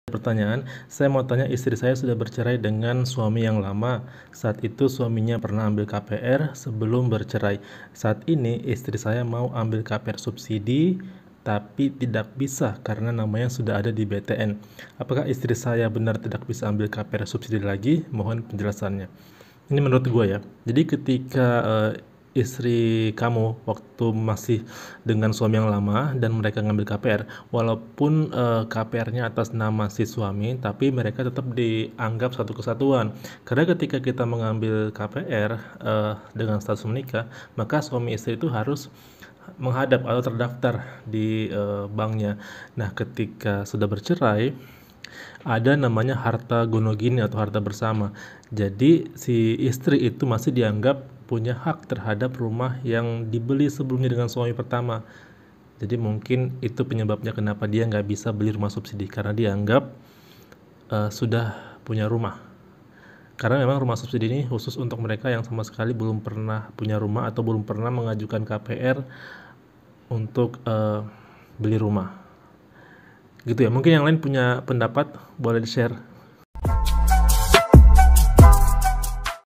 Mungkin Karena Gono-Gini Makanya Gak Bisa Beli Rumah Subsidi
Harta bersama atau harta gono-gini diperoleh setelah pasangan resmi menikah (selama perkawinan), seperti tanah, rumah, dan kendaraan, yang terhadap harta bersama tersebut, suami atau istri dapat bertindak atas persetujuan kedua belah pihak.